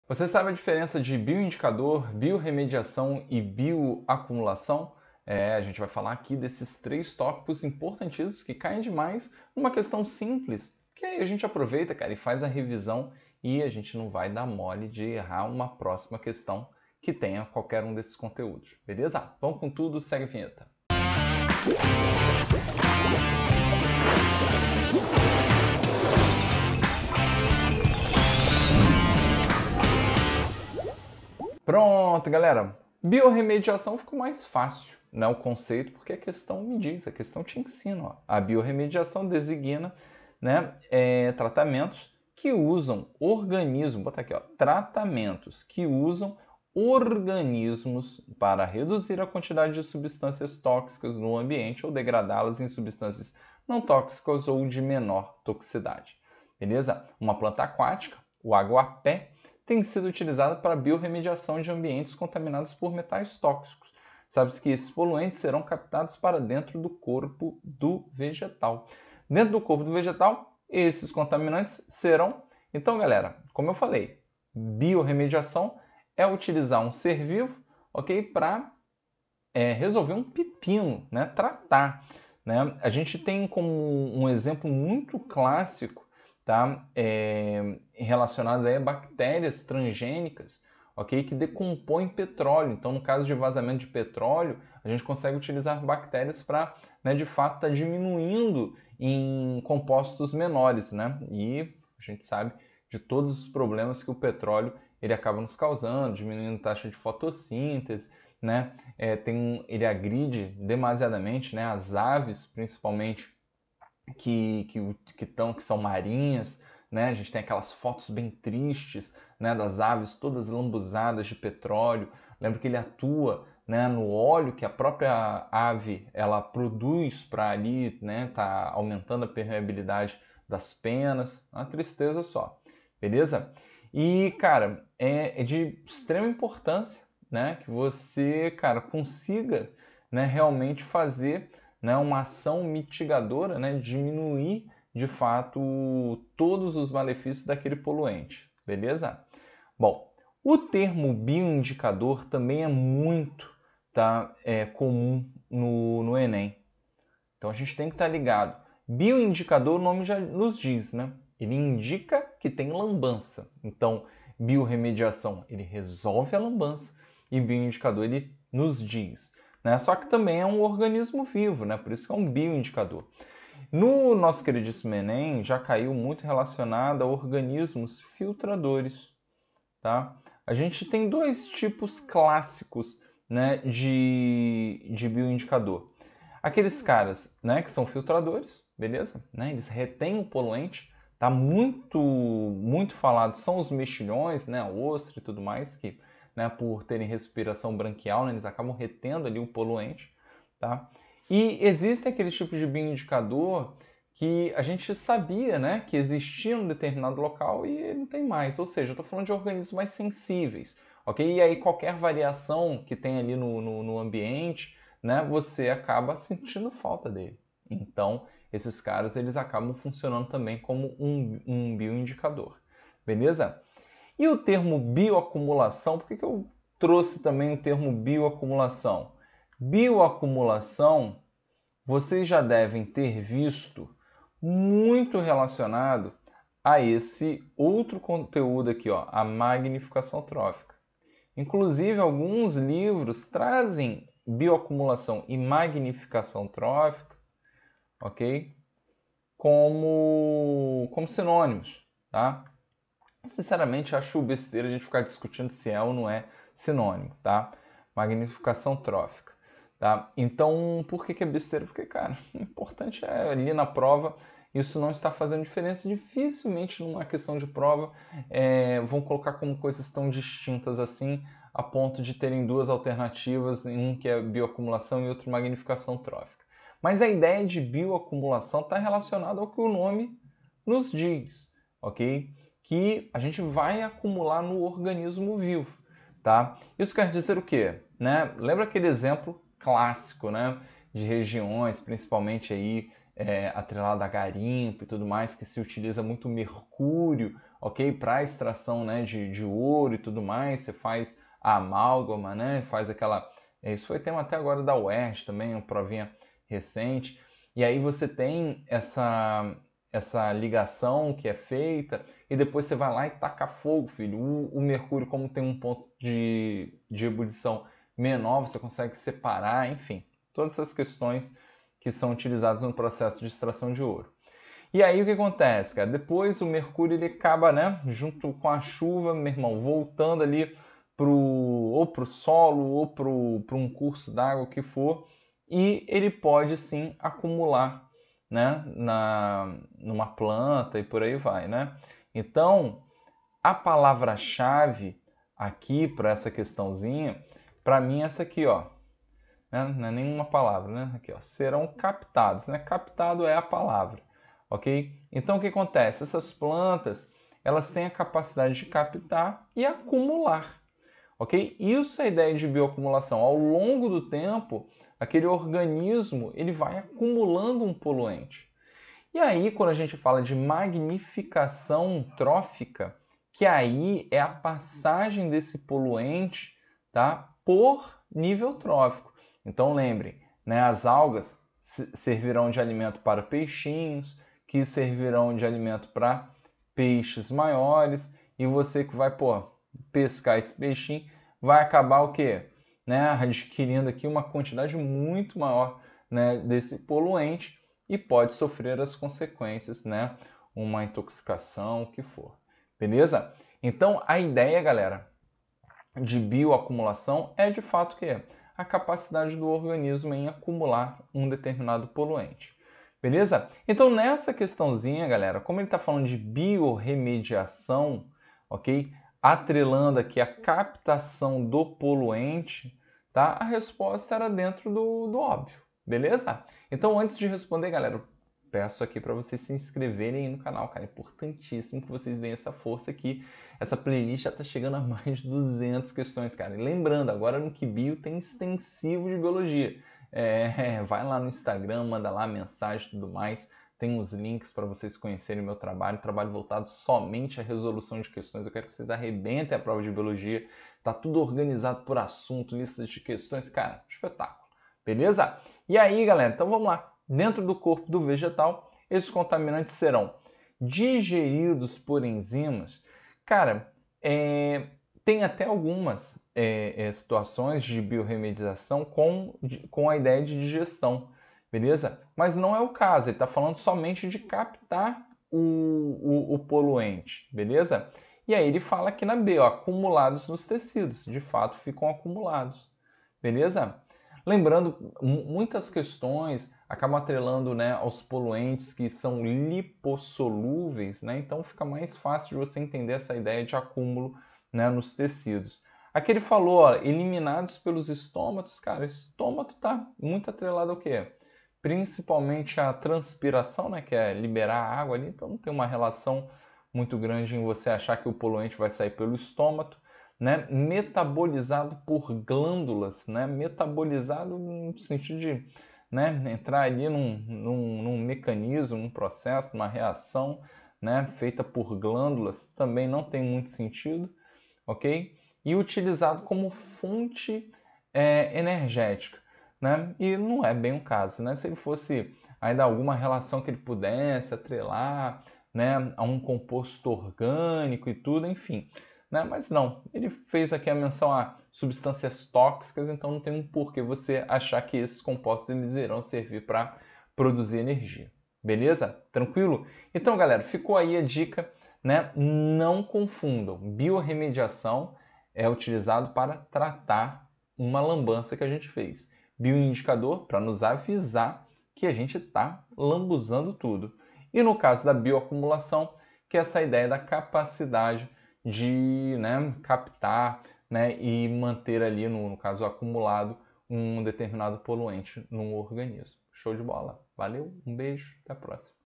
ENEM 2023 - A biorremediação designa tratamentos que usam organismos para reduzir a quantidade de
🔎LINKS DAS QUESTÕES SUGERIDAS NO VÍDEO

A biorremediação designa tratamentos que usam organismos para reduzir a quantidade de substâncias tóxicas no ambiente ou degradá-las em substâncias não tóxicas ou de menor toxicidade. Uma planta aquática,
o aguapé, tem sido utilizada para a biorremediação de
ambientes contaminados por metais tóxicos. Sabe-se
que esses poluentes serão captados para dentro do
corpo do vegetal.
Dentro do corpo do vegetal, esses contaminantes serão
A digeridos por enzimas.
B acumulados nos tecidos.
C eliminados pelos estômatos.
D metabolizados por glândulas.
E utilizados como fonte energética.